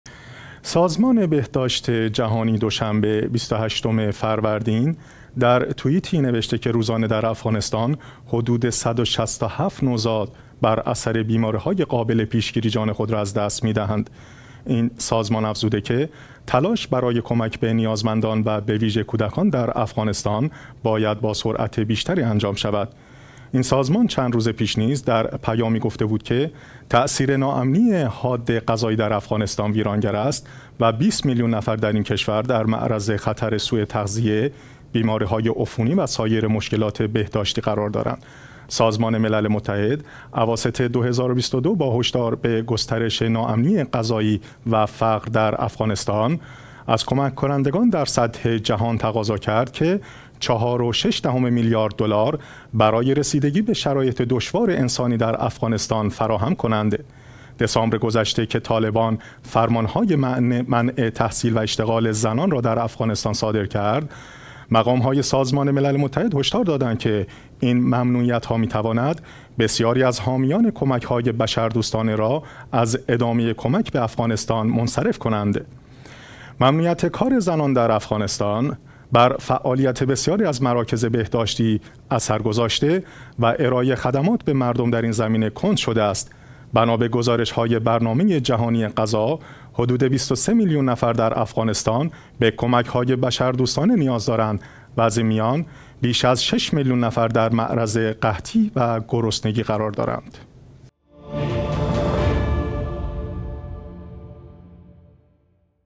سازمان بهداشت جهانی می‌گوید روزانه ۱۶۷ نوزاد در افغانستان می‌میرند
سازمان بهداشت جهانی دوشنبه، ۲۸ فروردین، در توییتی نوشته که روزانه در افغانستان حدود ۱۶۷ نوزاد بر اثر بیماری‌های قابل‌ پیشگیری جان‌ خود را از دست می‌دهند. این سازمان افزوده که تلاش‌ برای کمک به نیازمندان و به ویژه کودکان در افغانستان، باید با سرعت بیشتری انجام شود.
این سازمان چند روز پیش نیز در پیامی گفته بود که تاثیر ناامنی حاد غذایی در افغانستان ویرانگر است و ۲۰ میلیون نفر در این کشور در معرض خطر سوء تغذیه، بیماری‌های عفونی و سایر مشکلات بهداشتی قرار دارند.
سازمان ملل متحد اواسط ۲۰۲۲ با هشدار به گسترش ناامنی غذایی و فقر در افغانستان، از کمک‌ کنندگان در سطح جهان تقاضا کرد که ۴.۶ میلیارد دلار برای رسیدگی به شرایط دشوار انسانی در افغانستان فراهم کنند.
دسامبر گذشته که طالبان فرمان‌های منع تحصیل و اشتغال زنان را در افغانستان صادر کرد، مقام‌های سازمان ملل متحد هشدار دادند که این ممنوعیت‌ها می‌تواند بسیاری از حامیان کمک‌های بشردوستانه را از ادامه ى کمک به افغانستان منصرف کنند.
ممنوعیت کار زنان در افغانستان، بر فعالیت بسیاری از مراکز بهداشتی اثر گذاشته و ارایه خدمات به مردم در این زمینه کند شده است.
بنا به گزارش‌های برنامه جهانی غذا، حدود ۲۳ میلیون نفر در افغانستان به کمک هاى بشردوستانه نیاز دارند و از این میان، بیش از شش میلیون نفر در معرض قحطی و گرسنگی قرار دارند.